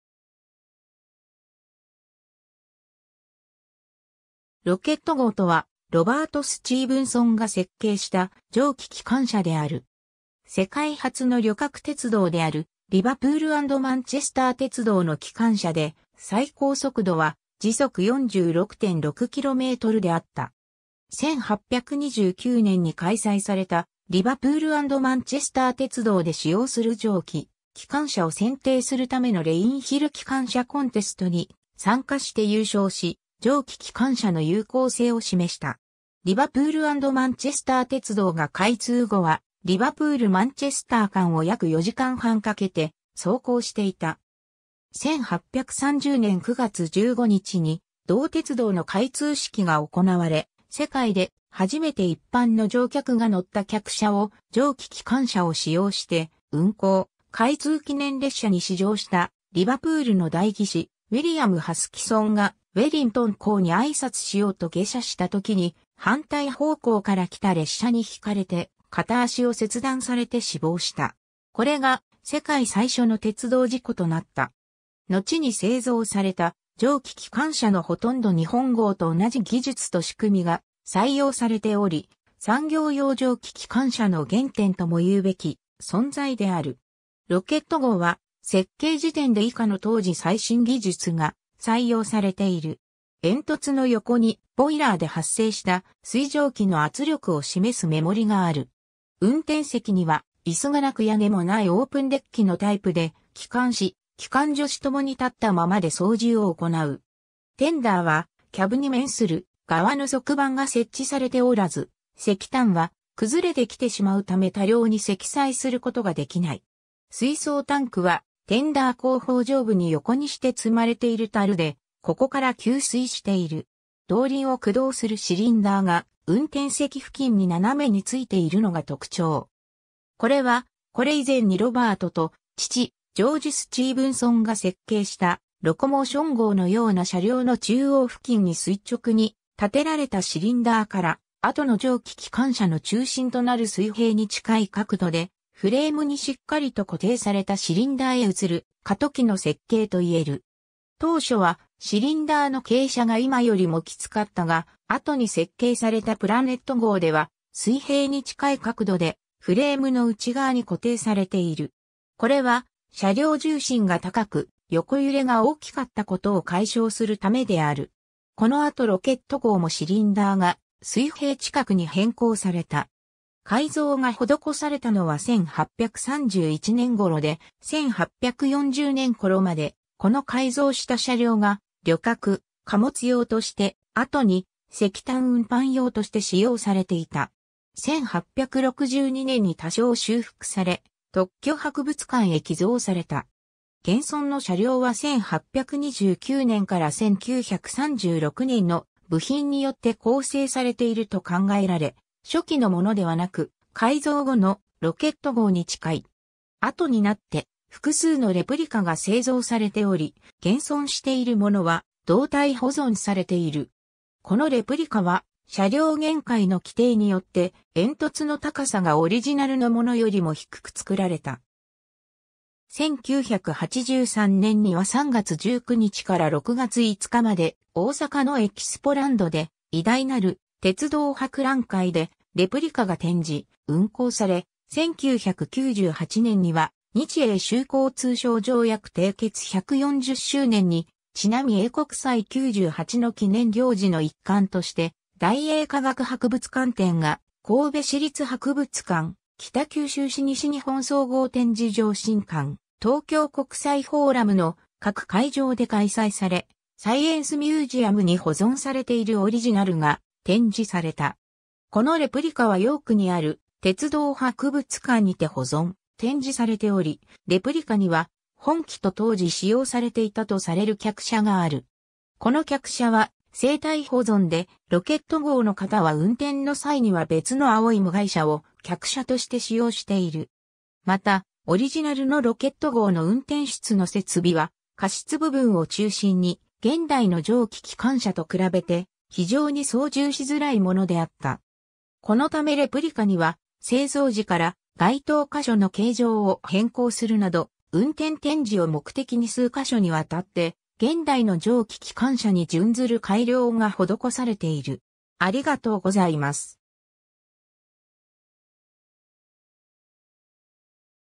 ロケット号
リバプール・アンド・マンチェスター鉄道の機関車
ロケット号（ロケットごう）とは、ロバート・スチーブンソン が設計した蒸気機関車である。
世界初の旅客鉄道であるリバプール・アンド・マンチェスター鉄道の機関車で、最高速度は時速46.6kmであった。
 1829年に開催された、リバプール・アンド・マンチェスター鉄道で使用する蒸気機関車を選定するためのレインヒル機関車コンテスト（レインヒル・トライアル）に参加して優勝し、蒸気機関車の有効性を示した。
 リバプール・アンド・マンチェスター鉄道が開通後はリヴァプール-マンチェスター間を約四時間半かけて走行していた。
 1830年9月15日に同鉄道の開通式が行われ、世界で初めて一般の乗客が乗った客車を蒸気機関車を使用して運行、開通記念列車に試乗したリヴァプールの代議士ウィリアム・ハスキソンがウェリントン公（当時首相）に挨拶しようと下車したときに、反対方向から来た列車に轢かれて片足を切断されて死亡した。
これが世界最初の鉄道事故となった。
 のちに製造された蒸気機関車のほとんどに本号と同じ技術と仕組みが採用されており、産業用蒸気機関車の原点ともいうべき存在である。
 ロケット号は設計時点で以下の当時最新技術が採用されている。
 煙突の横にボイラーで発生した水蒸気の圧力を示す目盛がある。
運転席には椅子がなく屋根もないオープンデッキのタイプで、機関士、機関助士共に立ったままで操縦を行う。
テンダーはキャブに面する側の側板が設置されておらず、石炭は崩れてきてしまう為多量に積載することが出来ない。
水槽タンクはテンダー後方上部に横にして積まれている樽で、ここから給水している。
 動輪を駆動するシリンダーが、運転席付近に斜めについているのが特徴。
これは、これ以前にロバートと父ジョージ・スチーブンソンが設計したロコモーション号のような車両の中央付近に垂直に立てられたシリンダーから、後の蒸気機関車の中心となる水平に近い角度でフレームにしっかりと固定されたシリンダーへ移る過渡期の設計といえる。
当初はシリンダーの傾斜が今よりもきつかったが、後に設計されたプラネット号では、水平に近い角度でフレームの内側に固定されている。
これは車両重心が高く横揺れが大きかったことを解消するためである。
この後ロケット号もシリンダーが水平近くに変更された。
改造が施されたのは1831年頃で、1840年頃までこの改造した車両が旅客・貨物用として、後に石炭運搬用として使用されていた。
1862年に多少修復され特許博物館（のちのサイエンス・ミュージアム）へ寄贈された。
現存の車両は1829年～1936年の部品によって構成されていると考えられ、初期のものではなく、改造後の「ロケット号」に近い。
 後になって複数のレプリカが製造されており、現存しているものは動態保存されている。
このレプリカは車両限界の規定によって煙突の高さがオリジナルの物よりも低く作られた。
1983年には3月19日から6月5日まで大阪のエキスポランドで『偉大なる鉄道博覧会』でレプリカが展示、運行され、1998年には 『日英修好通商条約』締結140周年にちなみ英国祭98の記念行事の一環として大英科学博物館展が神戸市立博物館（3月21日-5月17日）、北九州市西日本総合展示場新館（5月31日-7月12日）、東京国際フォーラム（7月22日-8月30日）の各会場で開催され、サイエンス・ミュージアムに保存されているオリジナルが展示された。
このレプリカはヨークにある鉄道博物館にて保存、展示されており、レプリカには本機と当時使用されていたとされる客車がある。
この客車は静態保存で、ロケット号の方は運転の際には別の青い無蓋車を客車として使用している。
 また、オリジナルのロケット号の運転室の設備は火室部分を中心に現代の蒸気機関車と比べて非常に操縦しづらいものであった。
このためレプリカには製造時から該当箇所の形状を変更するなど、運転展示を目的に数ヶ所にわたって現代の蒸気機関車に準ずる改良が施されている。